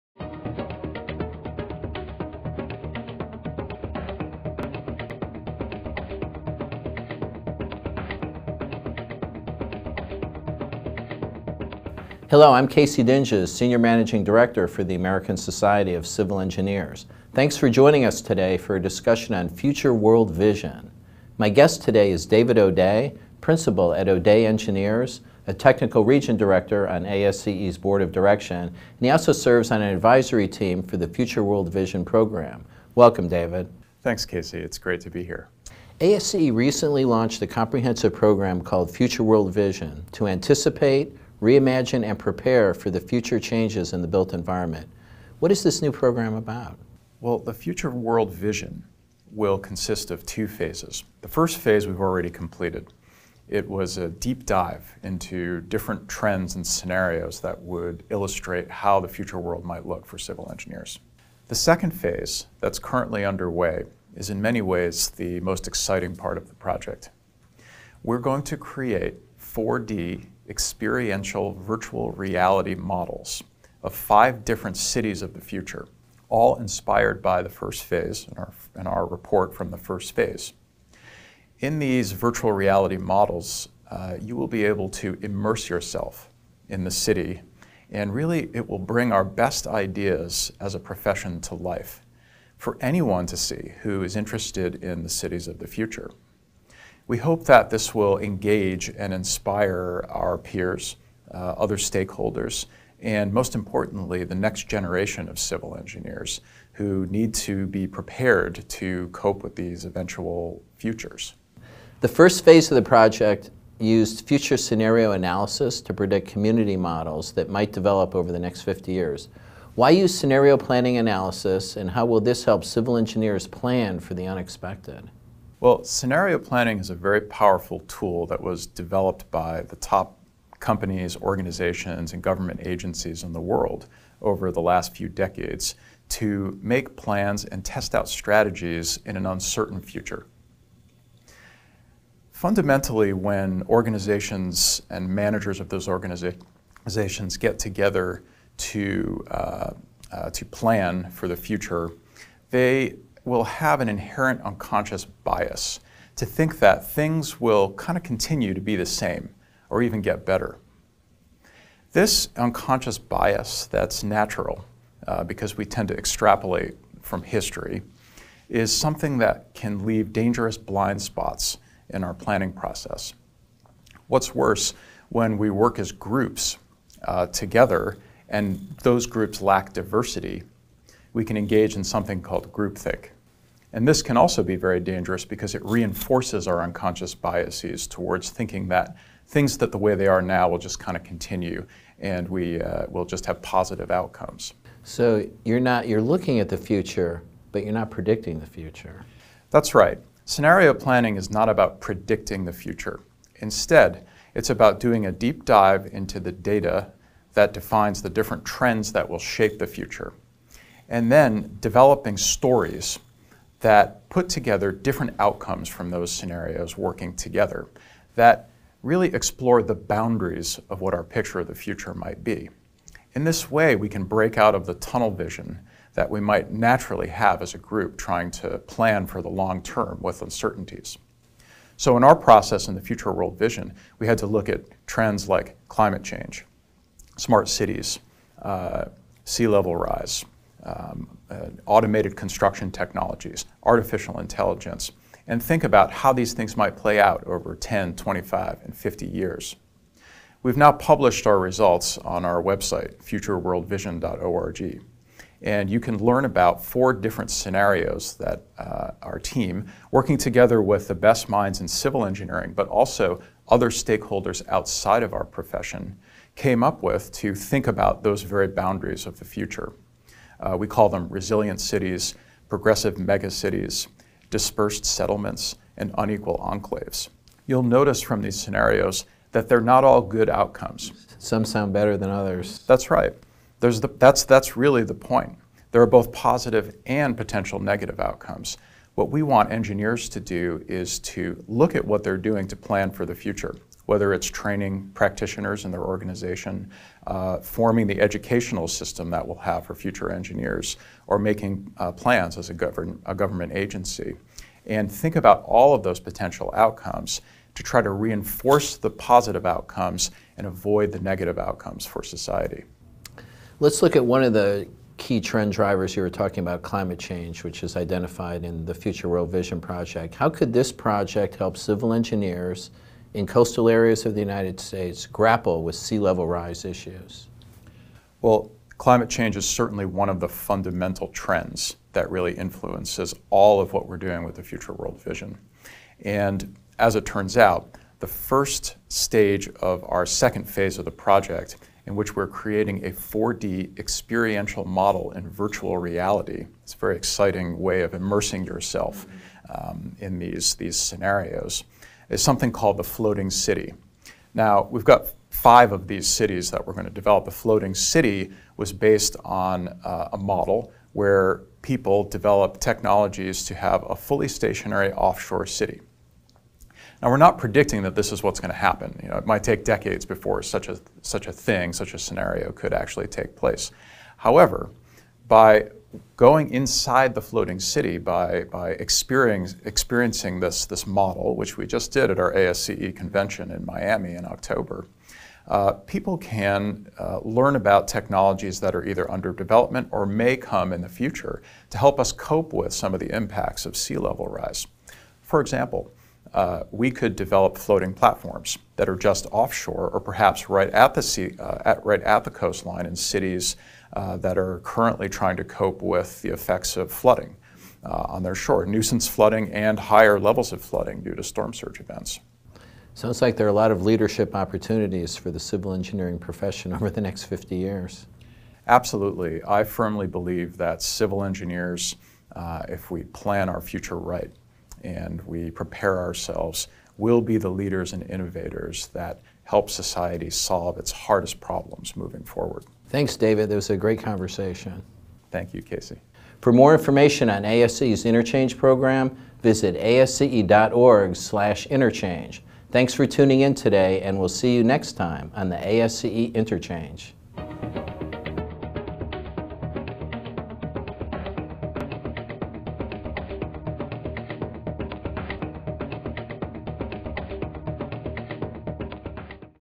Experience Tomorrow’s Built Future Today
Imagine the world 50 years from now. What does 2070 look like? How is society continuing to function? How have technological and environmental challenges affected our cities? ASCE’s Future World Vision brings those possibilities to fruition.

In this episode of the “ASCE Interchange,” David Odeh, principal of Odeh Engineers, discusses the comprehensive Future World Vision program and how it helps civil engineers strategize for an uncertain future.